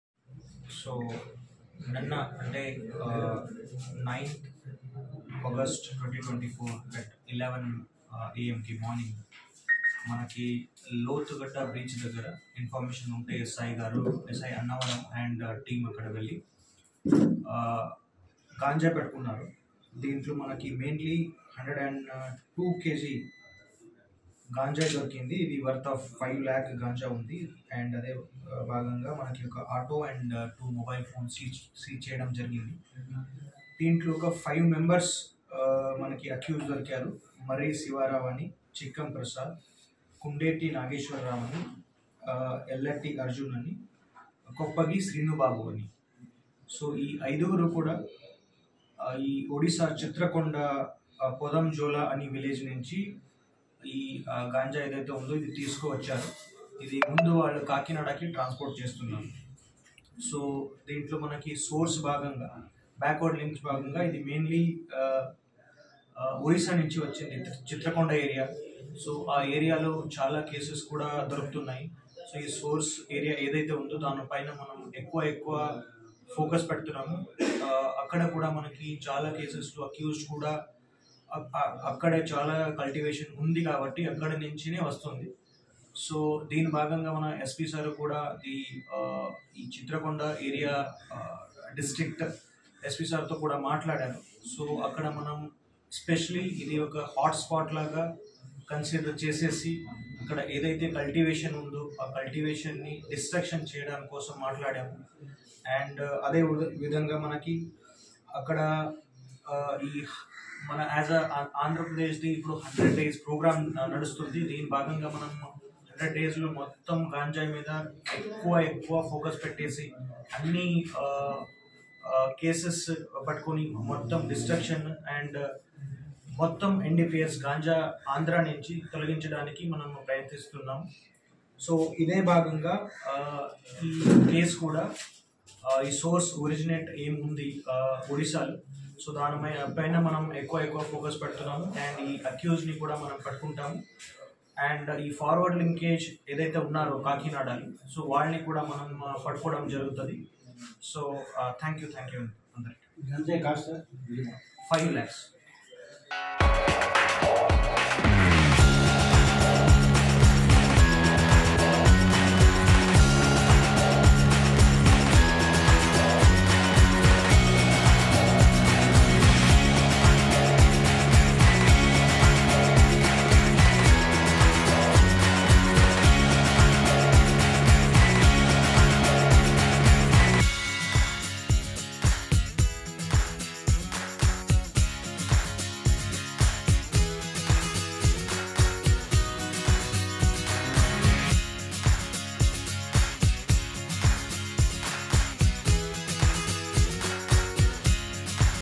అన్నవరం లో 200 కిలోల గంజాయి పట్టివేత | 4 గురు అరెస్ట్ | AP News
TopTeluguTv#200 kg of ganja seized in Annavaram police